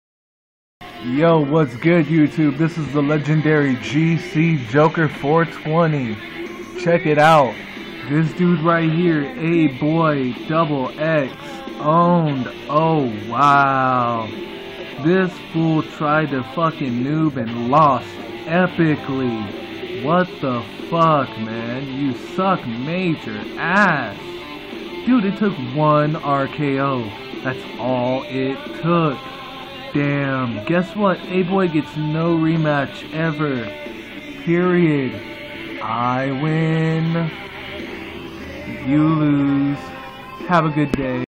Aboii owned.... no rematch
Aboii got hit with one RKO and lost. Smmfh. No rematch